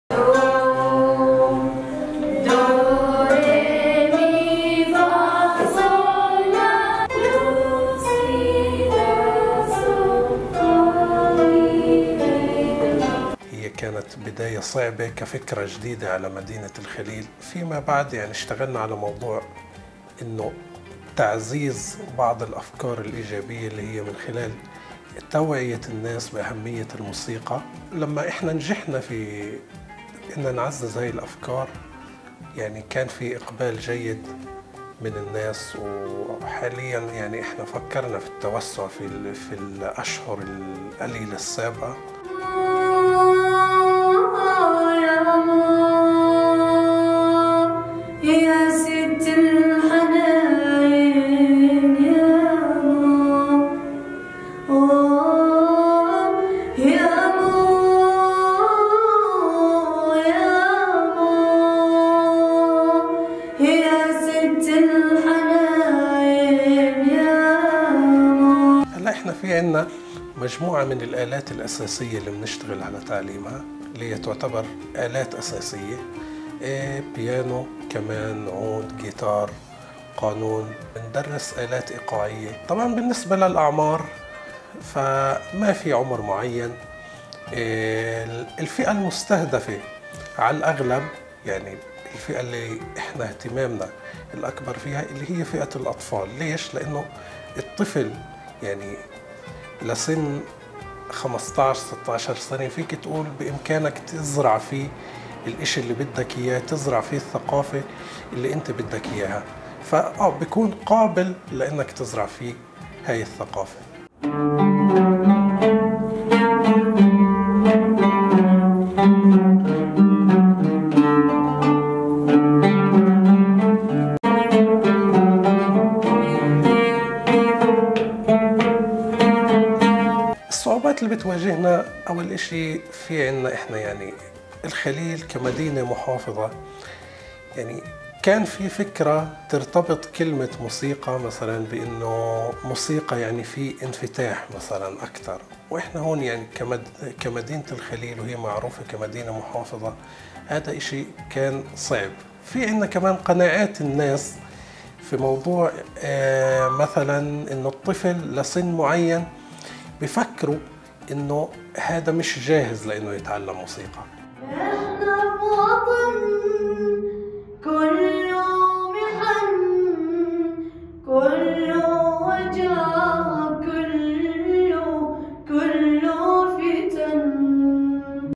مدرسة راست... أول مدرسة لتعليم الموسيقى في مدينة الخليل
الحدث: مدرسة راست... أول مدرسة لتعليم الموسيقى في مدينة الخليل

تصوير: مصعب شاور
مونتاج: إيهاب عاروري

Al Hadath صحيفة الحدث الفلسطيني

صحيفة الحدث الفلسطيني، صحيفة شهرية اقتصادية اجتماعية ثقافية، تصدر عن شركة الحدث للإعلام والطباعة والنشر، بيرزيت\رام الله\ فلسطين